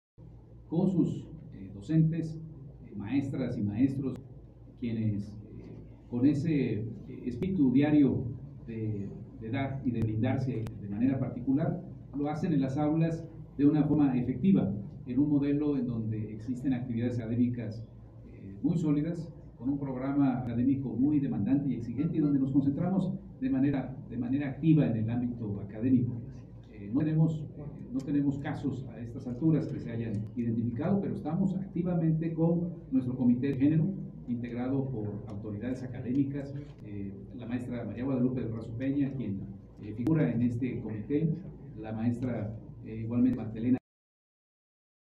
Roberto Mendoza, director de la Escuela Libre de Derecho, da mensaje sobre la institución
Roberto Mendoza, director de la Escuela Libre de Derecho, asegura que están libres de acoso sexual en el instituto, tras el caso del profesor que violentó a Camila Castelán.